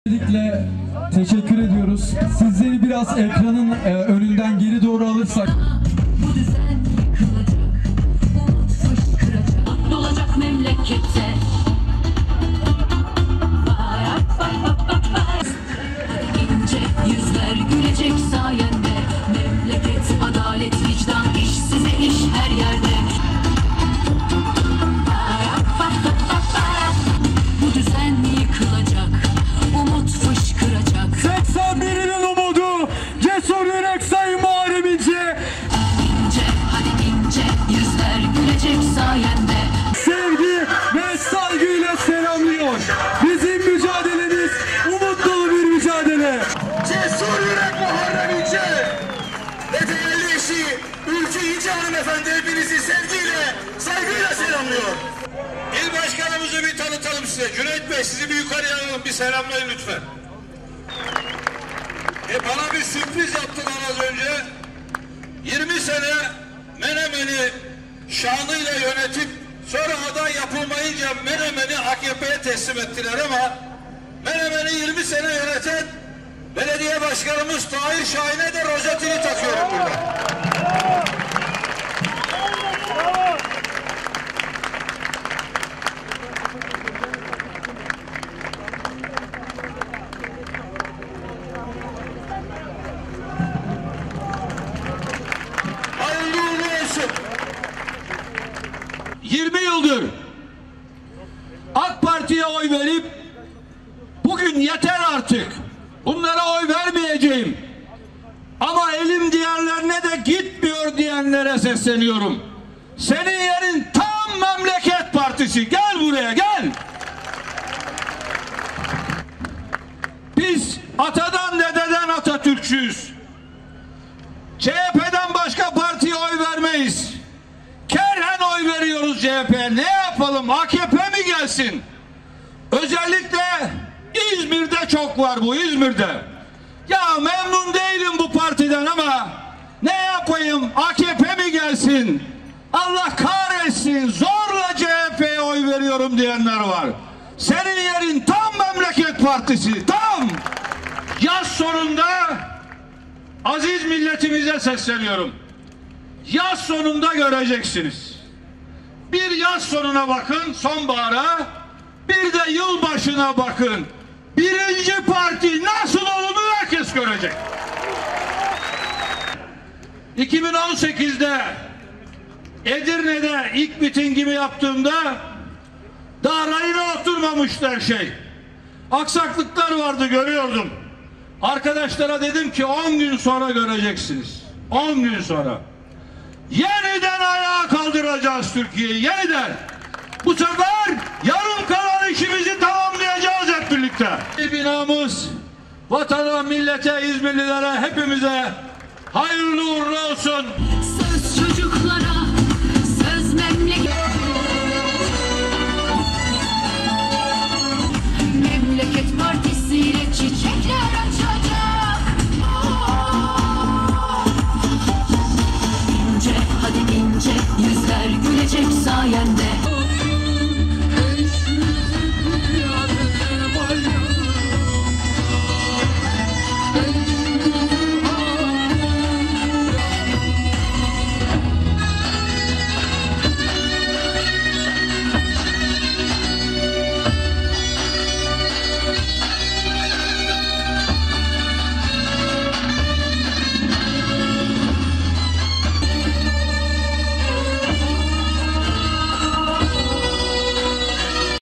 "Türkiye'yi yeniden ayağa kaldıracağız"
Memleket Partisi Genel Başkanı Muharrem İnce, partisinin ilk il binası açılışını İzmir'de gerçekleştirdi. CHP'ye oy veren İzmirlileri partisinin saflarına davet eden İnce, "Türkiye'yi yeniden ayağa kaldıracağız. Bu sefer yarım kalan işimizi tamamlayacağız" dedi.